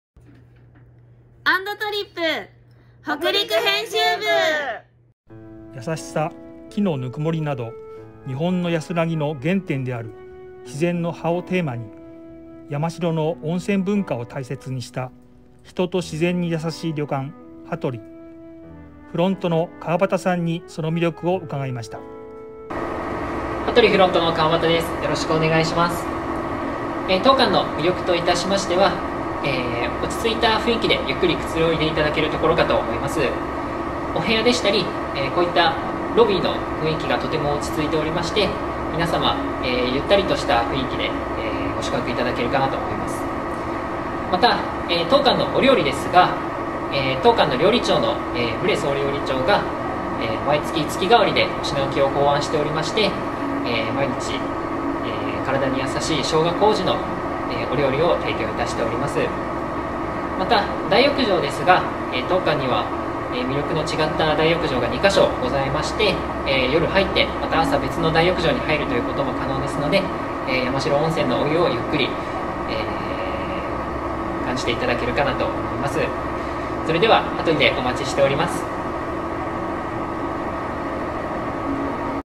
【山代温泉】葉渡莉フロント川端さんのおすすめは●●●を使ったお料理！
石川県加賀温泉郷。美食家北大路魯山人が愛した山代温泉にある旅館『葉渡莉』。
フロントの川端さんに魅力を教えていただきました！